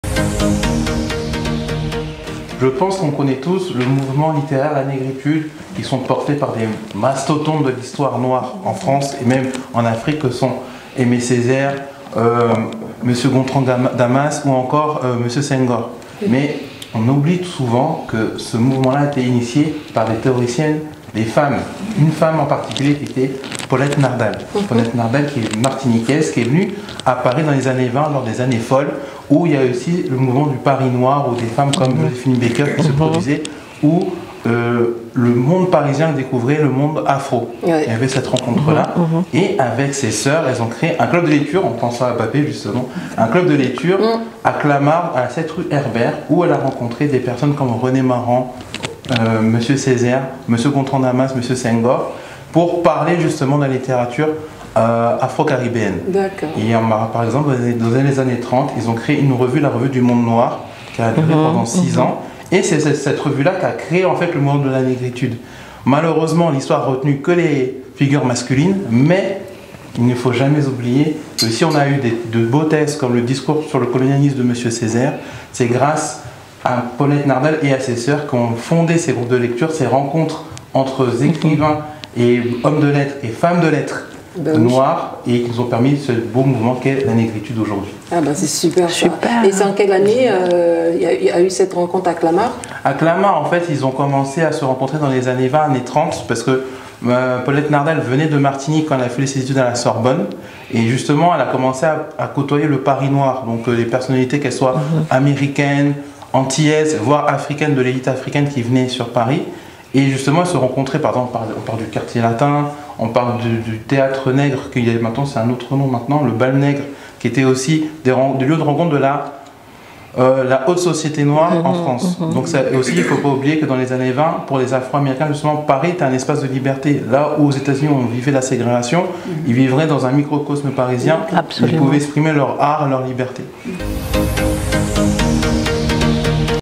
Un parcours inspirant raconté par Yoni : Paulette Nardal
Extrait de notre émission N 13, le parcours inspirant consacré à la Martiniquaise Paulette Nardal, journaliste, écrivaine, activiste, professeure d'anglais, marraine de la Négritude et musicienne .. Une chronique proposée par Yoni ( @_wakongo_ ) ..
11 h